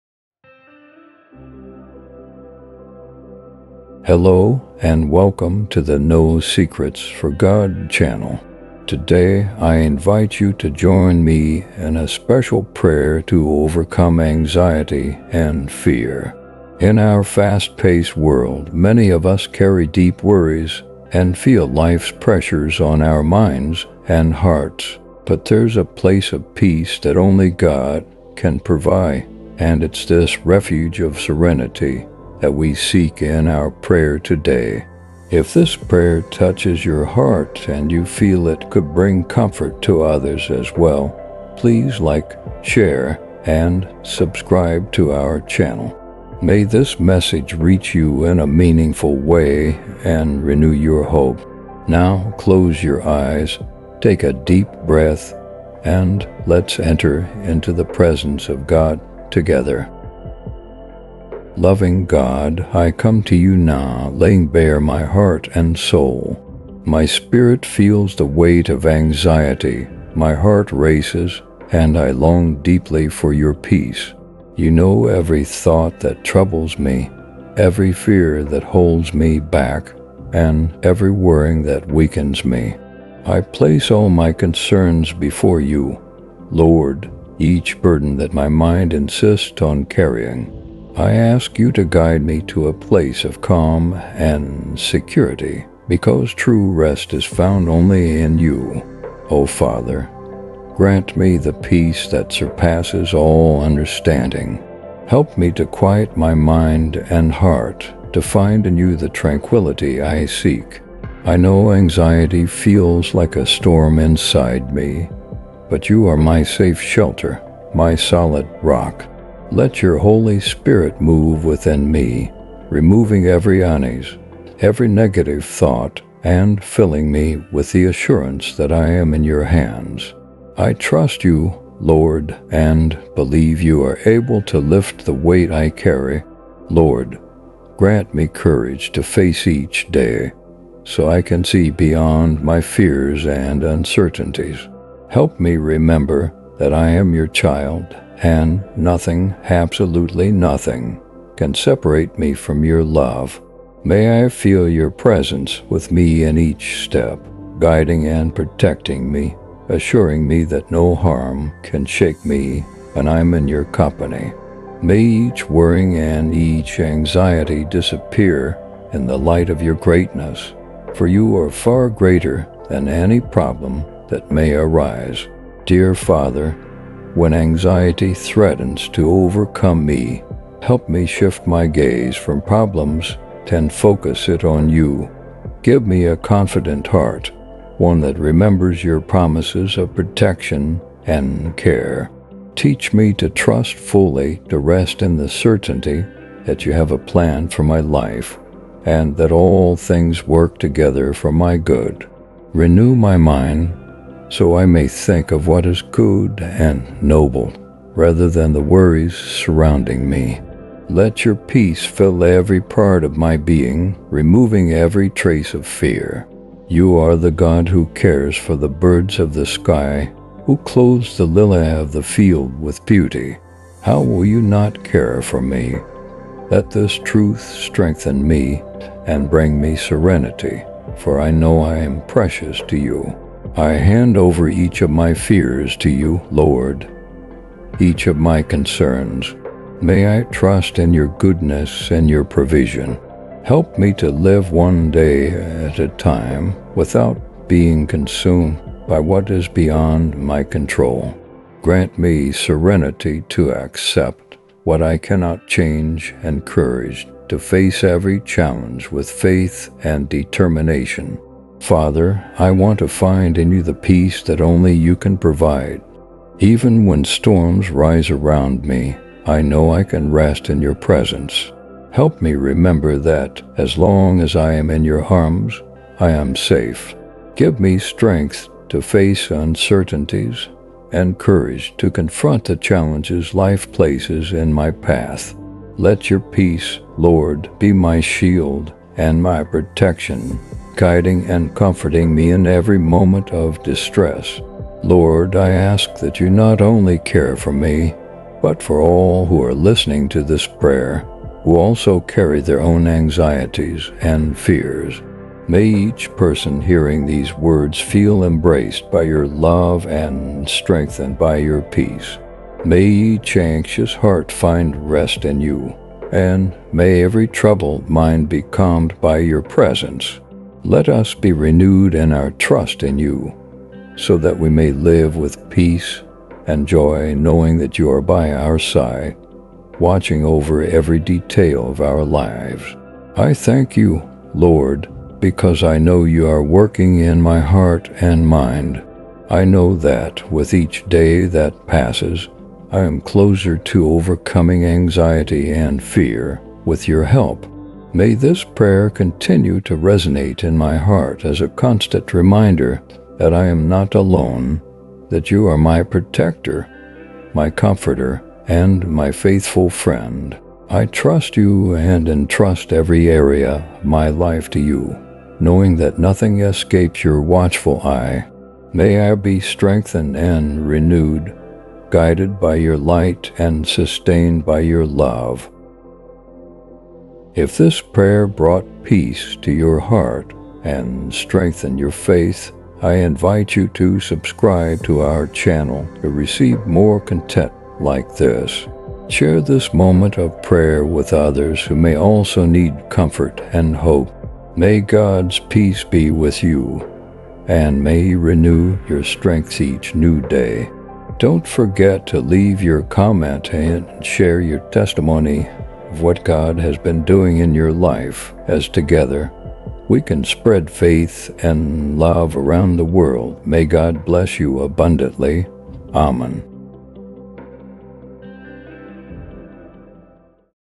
Prayer for Freedom: Overcoming Anxiety and Fear with God’s Strength
🙏Are you struggling with anxiety and fear? In this powerful prayer, we seek the comfort and peace that only God can provide. Join us for a moment of prayer that soothes the heart and renews faith, handing over every fear and worry to Him.
Let this message fill your life with hope and confidence, reminding you that in God, true serenity and strength are possible to overcome any challenge. Feel His divine presence and allow peace to fill your soul.🕊